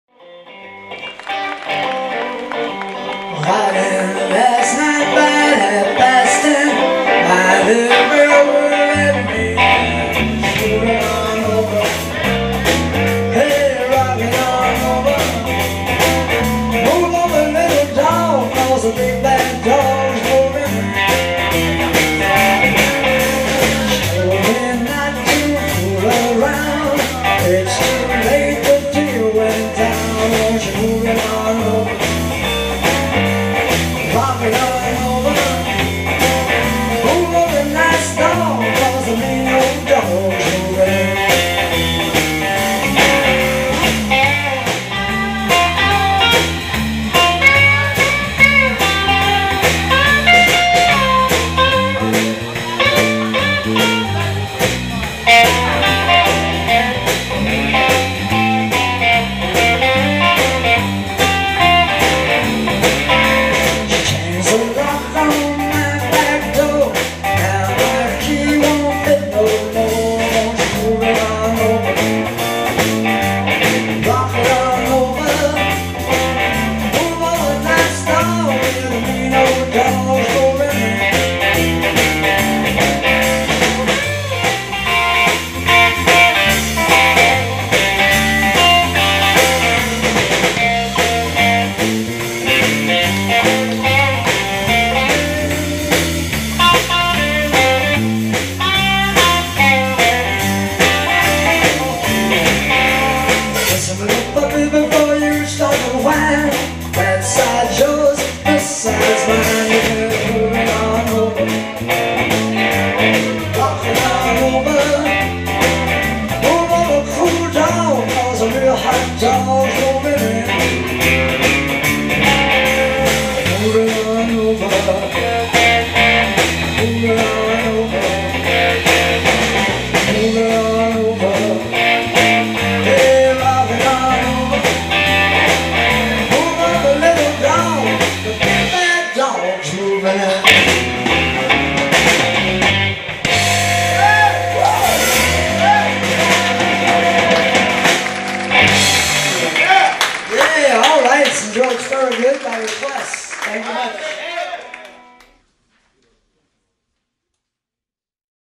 Howlin' Waters Band - Live @ The Post Oak Bar & Grill, La Grange, Texas!
Guitar, vocals - Howlin' Waters  //  Drums - Gary Ciuzio  //  Bass - St Louis Scot Vessell

Recorded live December 9, 2023 @, Post Oak B&G, La Grange, Texas.

Hank Williams cover.